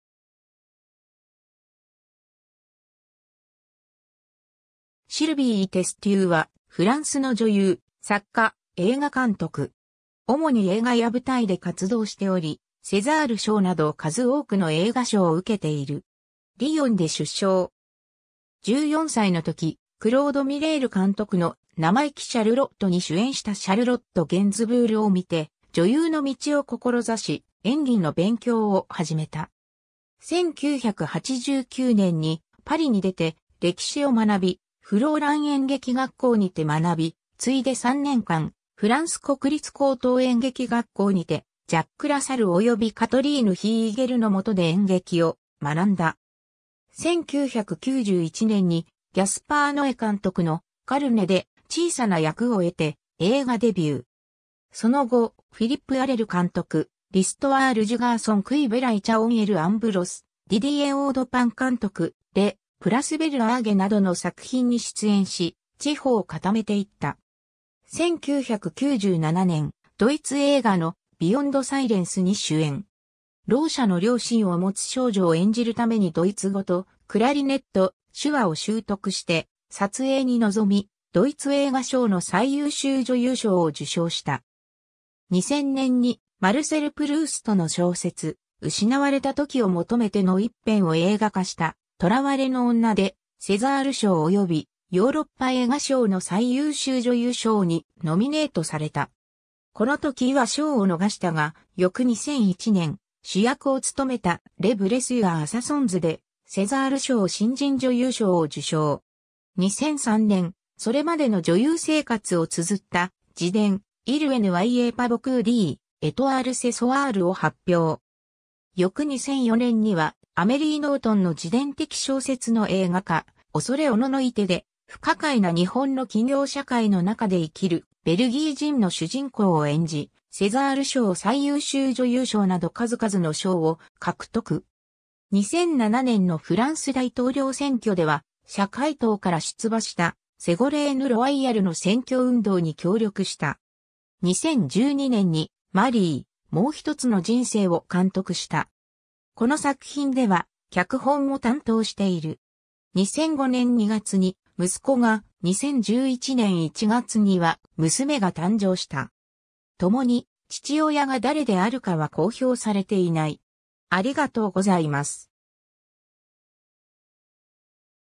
シルヴィー・テステュー
シルヴィー・テステュー（Sylvie Testud, 1971年1月17日 - ）は、フランスの女優、作家、映画監督。
主に映画や舞台で活動しており、セザール賞など数多くの映画賞を受けている。
 リヨンで出生。
14歳の時、クロード・ミレール監督の『なまいきシャルロット』に主演したシャルロット・ゲンズブールを見て女優の道を志し、演技の勉強を始めた。
1989年にパリに出て歴史を学び、フローラン演劇学校にて学び、次いで3年間、フランス国立高等演劇学校（コンセルヴァトワール）にて、ジャック・ラサルおよびカトリーヌ・ヒーゲルの元で演劇を学んだ。
1991年にギャスパー・ノエ監督の『カルネ』で小さな役を得て映画デビュー。
その後、フィリップ・アレル監督『L'histoire du garçon qui voulait qu'on l'embrasse』（1994年）、ディディエ・オードパン監督『Le plus bel âge』（1995年）などの作品に出演し、地歩を固めていった。
 1997年、ドイツ映画の『ビヨンド・サイレンス』（カロリーヌ・リンク監督）に主演。
ろう者の両親を持つ少女を演じるためにドイツ語とクラリネット、手話を習得して撮影に臨み、ドイツ映画賞の最優秀女優賞を受賞した。
 2000年にマルセル・プルーストの小説『失われた時を求めて』の一編を映画化した『囚われの女』（シャンタル・アケルマン監督）でセザール賞およびヨーロッパ映画賞の最優秀女優賞にノミネートされた。
この時は賞を逃したが、翌2001年、主役を務めた『Les blessures assassines』（ジャン＝ピエール・ドゥニ監督）でセザール賞新人女優賞を受賞。
 2003年、それまでの女優生活を綴った自伝『Il n'y a pas beaucoup d'étoiles ce soir』を発表。
翌2004年にはアメリー・ノートンの自伝的小説の映画化『畏れ慄いて』（アラン・コルノー監督）で、不可解な日本の企業社会の中で生きるベルギー人の主人公を演じ、セザール賞最優秀女優賞など数々の賞を獲得。
 2007年のフランス大統領選挙では、社会党から出馬したセゴレーヌ・ロワイヤルの選挙運動に協力した。
 2012年に『マリー、もうひとつの人生』を監督した。
この作品では脚本も担当している。
 2005年2月に息子が、2011年1月には娘が誕生した。
ともに父親が誰であるかは公表されていない。